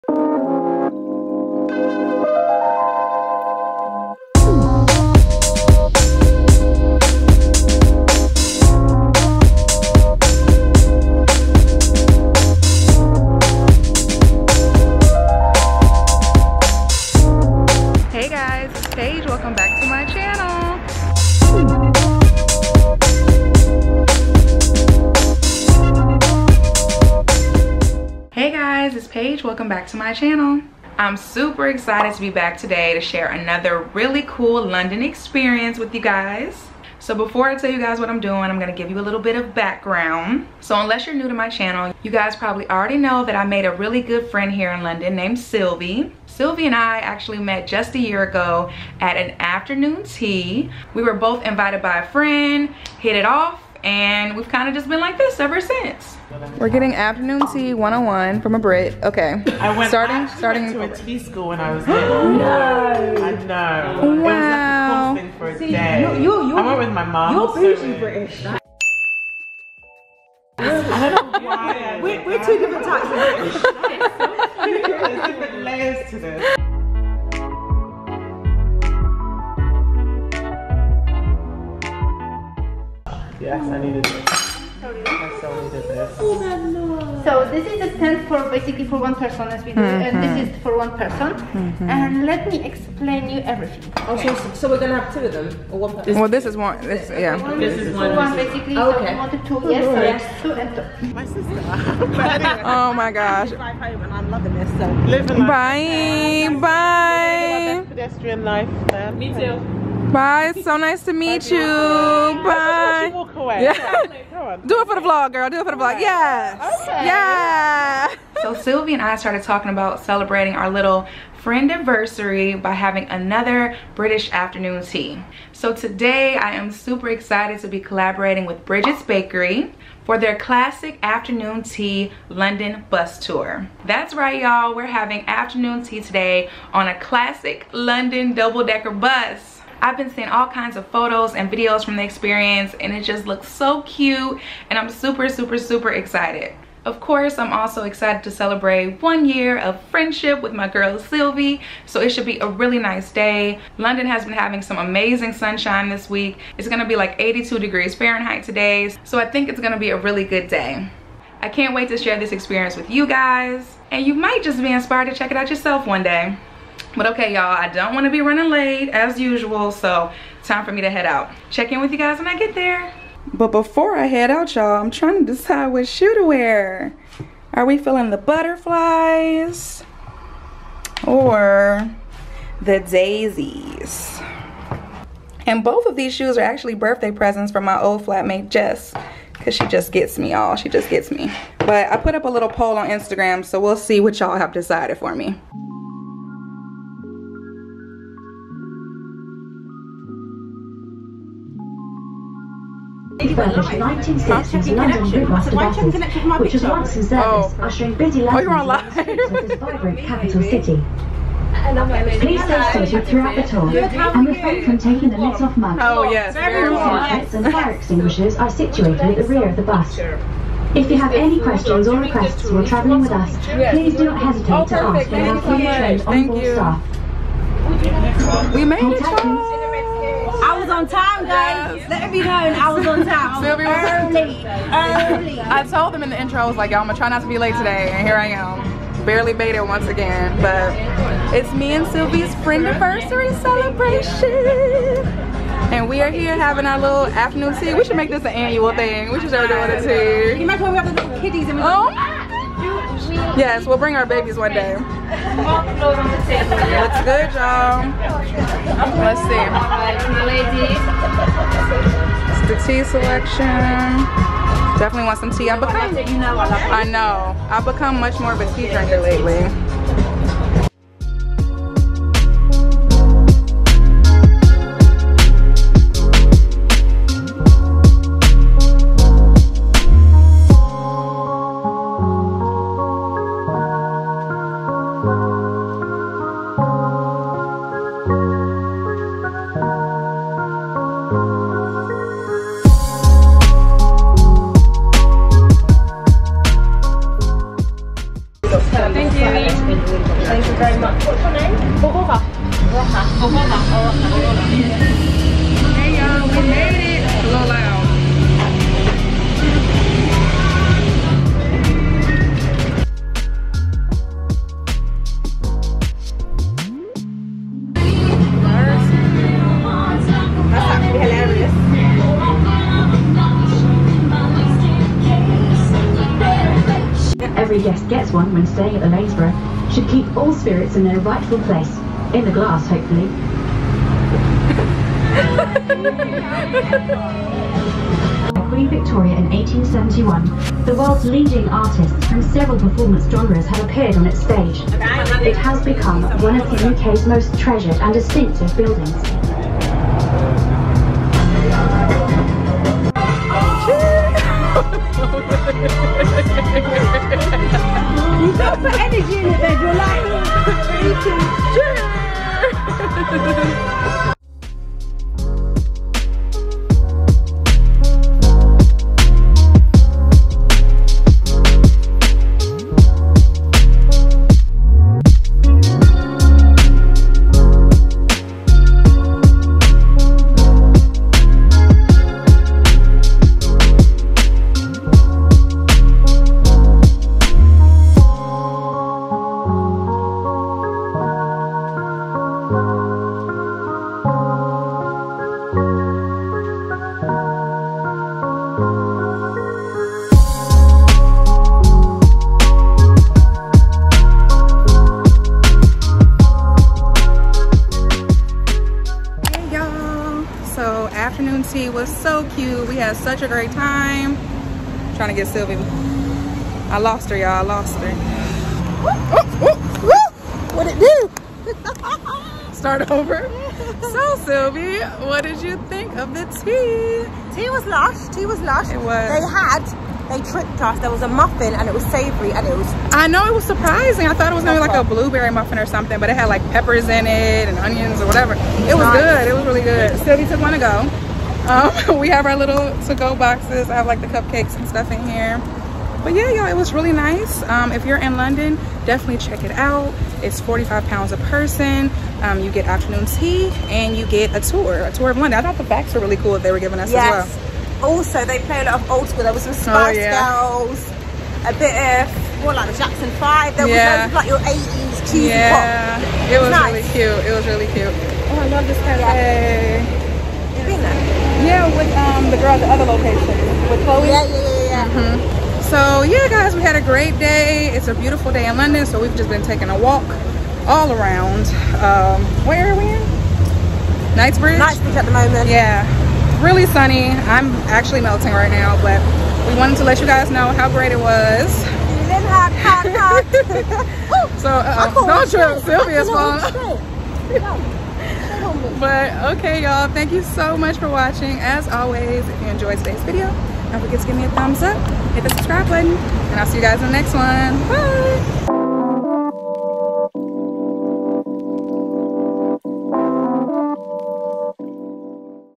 The CUTEST British Afternoon Tea in London! // London Vlog #87
My friend Sylvie and I recently celebrated our 1 year "friendaversary" on the Brigit's Bakery Classic Afternoon Tea London Bus Tour! We had such a great time and it was just the cutest experience ever! Would you check out this cool afternoon tea experience?

FTC: Gifted press experience.